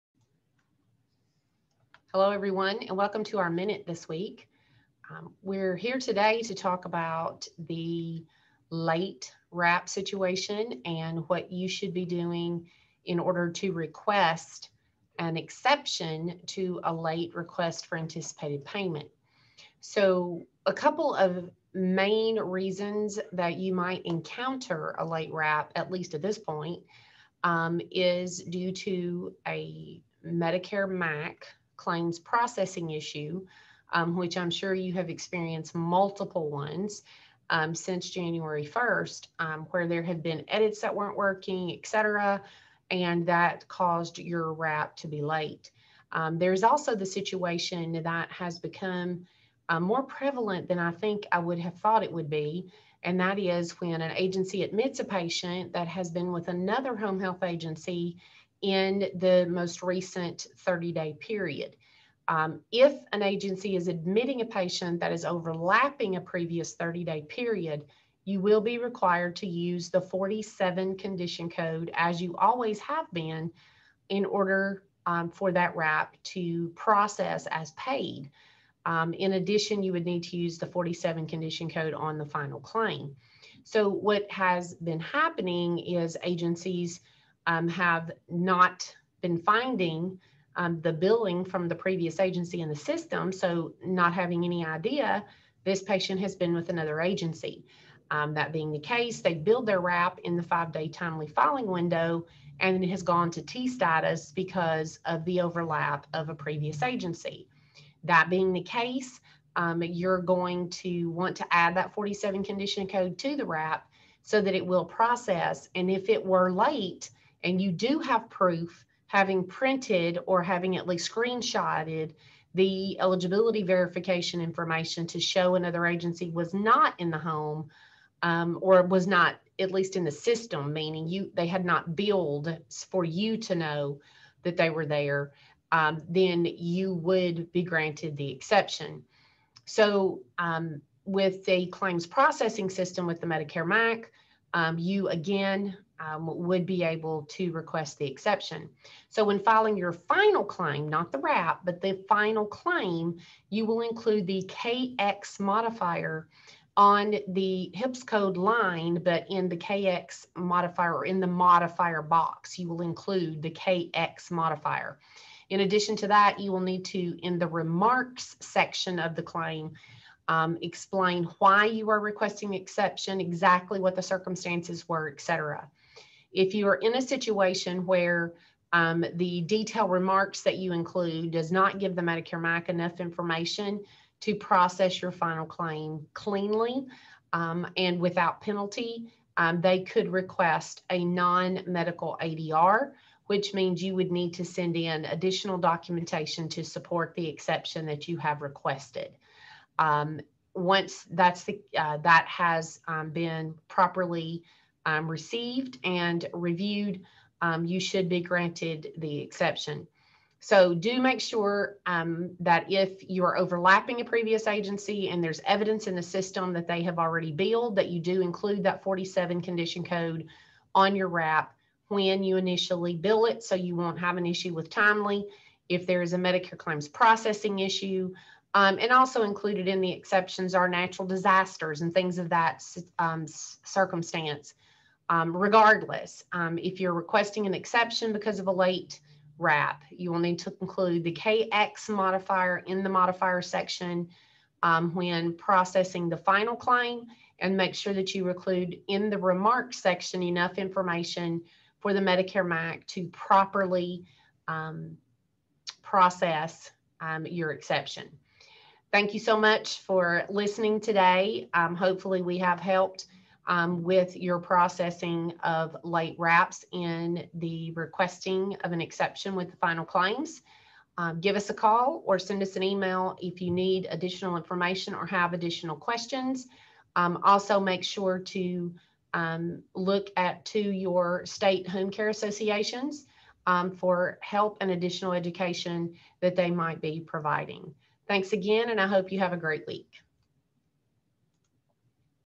Exceptions for Late RAPs!
This week’s video offers quick tips on ensuring that you are coding appropriately for requesting an exception to a late RAP in 2021. Today we are going to talk about the late RAP situation and what you should be doing to request an exception to a late request for anticipated payment.